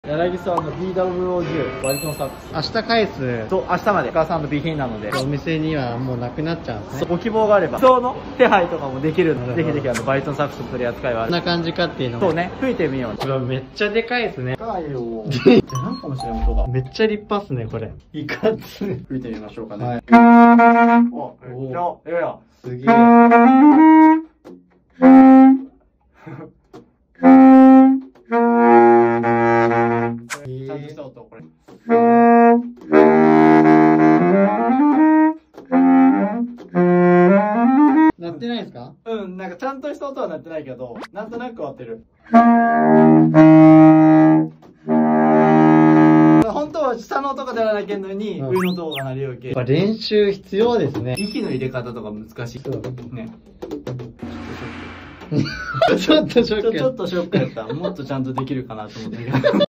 楽器店クラリネット奏者がバリトンサックスに挑戦してみた結果、、。
※試奏したバリトンサックスはメーカーへ返送済みです！
こんにちは！島村楽器直方店です！
本日は挑戦シリーズと題しまして、クラリネット奏者の加々見さんに
バリトンサックスに挑戦して頂きました👏👏👏

バリトンサックスを始めてみましたが、
でかさに驚きを隠せませんでしたねｗ
バリトンらしい重低音が素晴らしい楽器でした！

島村楽器直方店HPはこちら！

直方店管楽器HPはこちら！！

直方店Twitterはこちら！！！

撮影協力 : 加々見さん(管楽器担当)